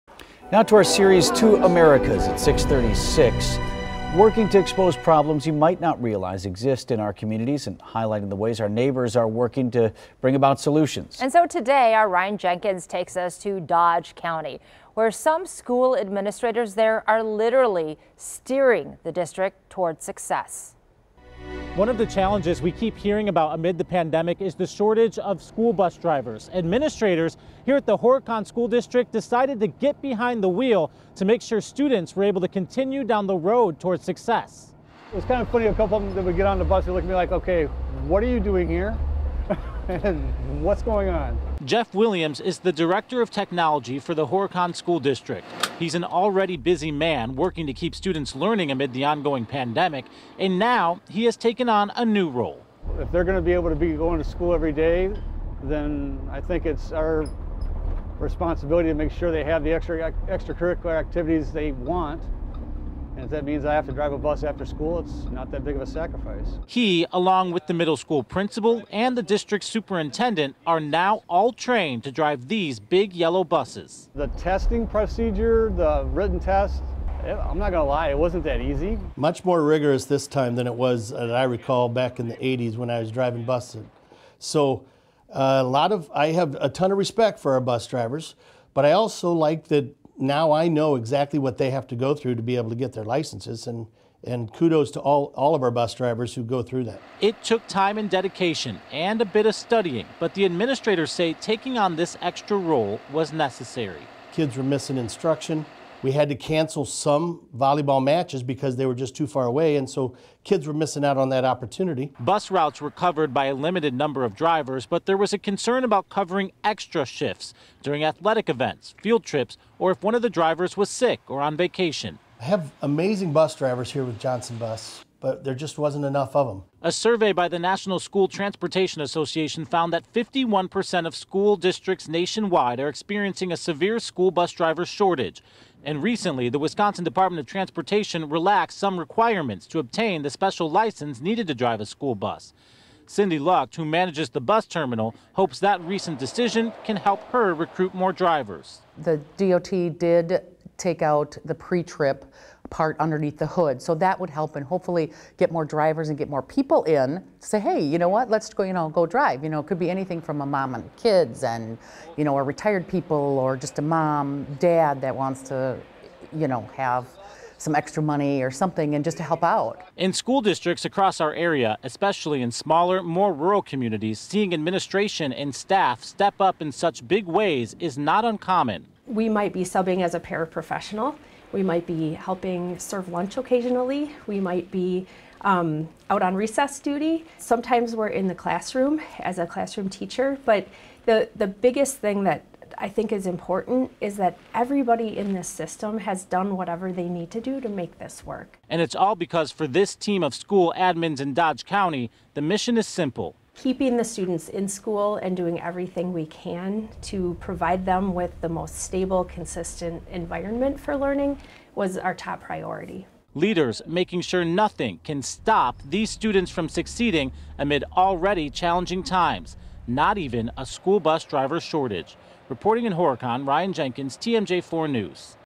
Horicon school administrators sub as school bus drivers amid driver shortage
In Dodge County, school district administrators are playing a role in combating the nationwide school bus driver shortage.